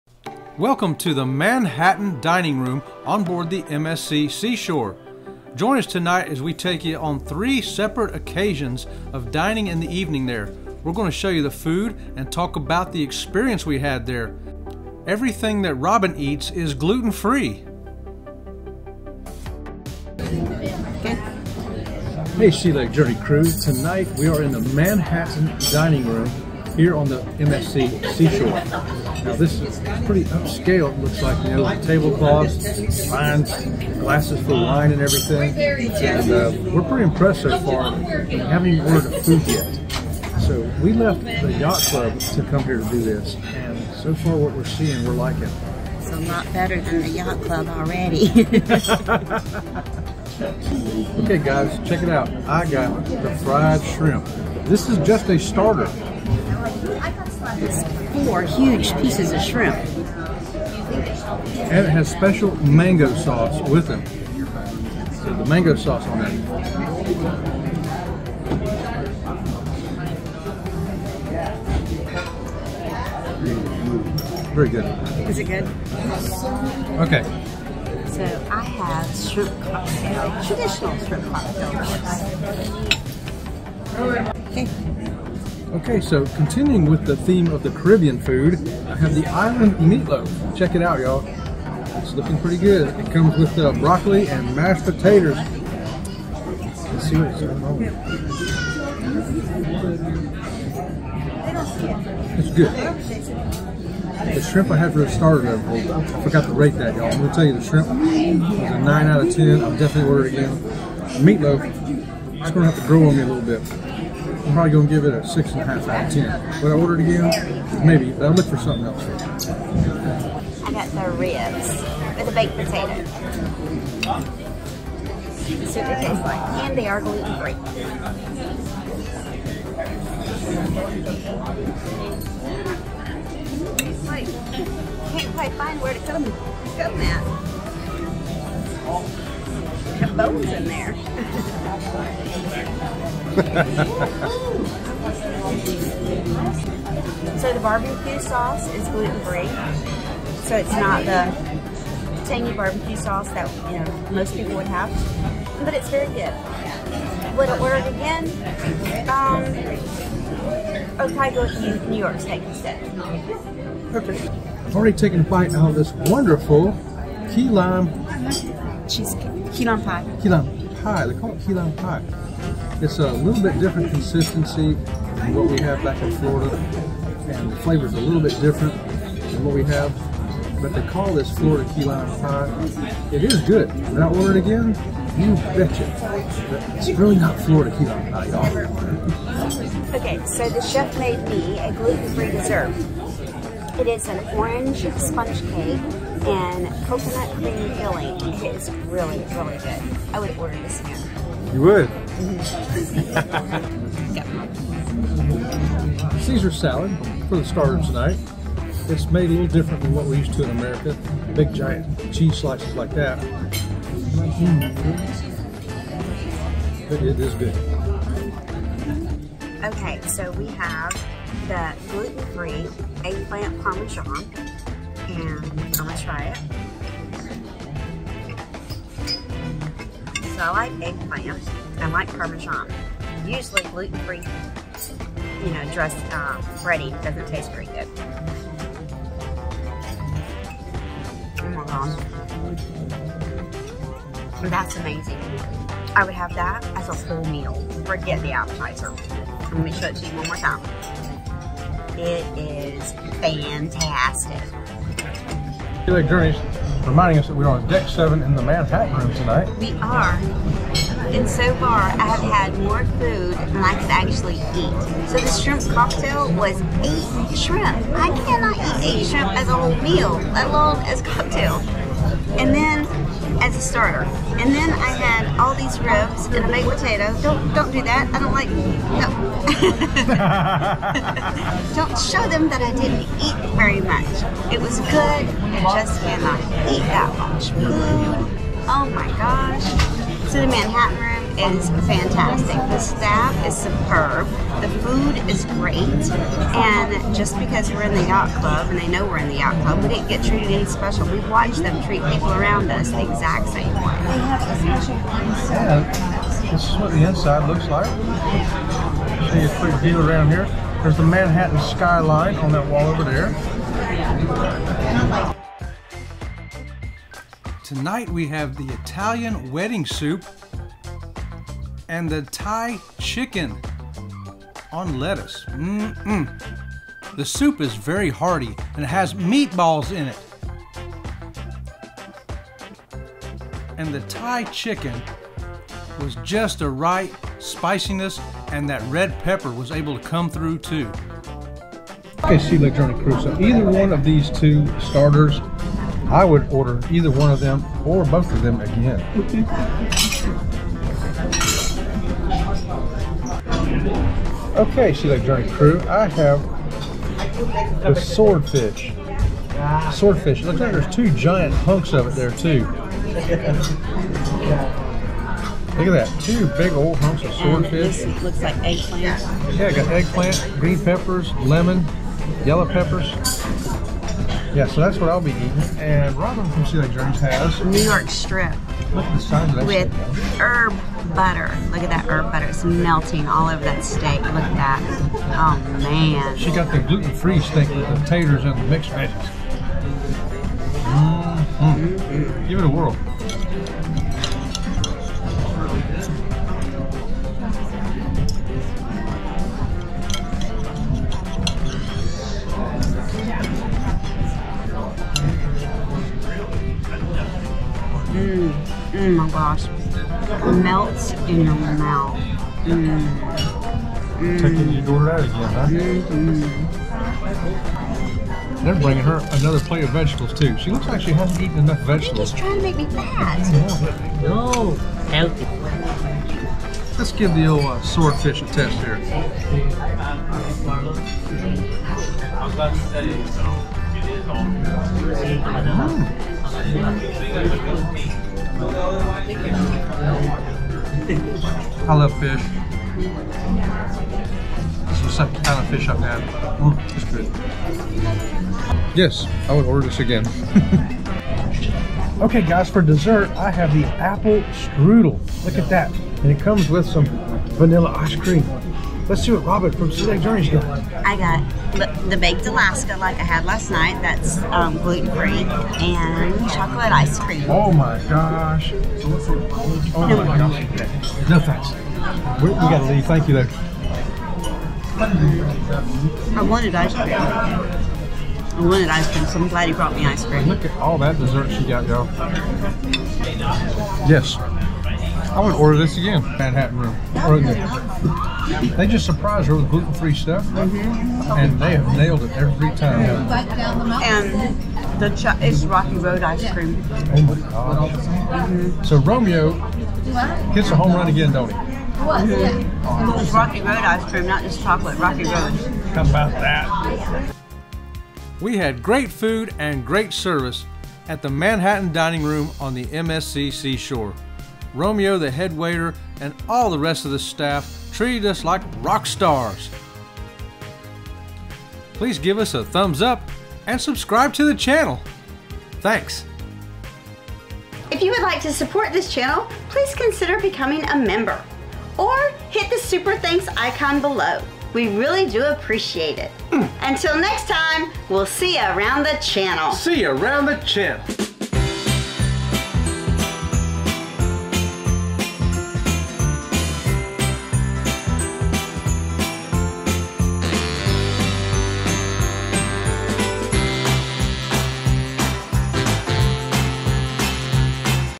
Insider tips for dining on MSC Seashore in Manhattan
Join Sea Leg Journeys as we share our experience in the Manhattan Dining Room on the MSC Seashore. This is a regular included dining room that is an anytime dining venue. We also share the Gluten-free dishes.

Become a Special Member of the Sea Leg Journey Crew.

Kathy Mills
Lori Barber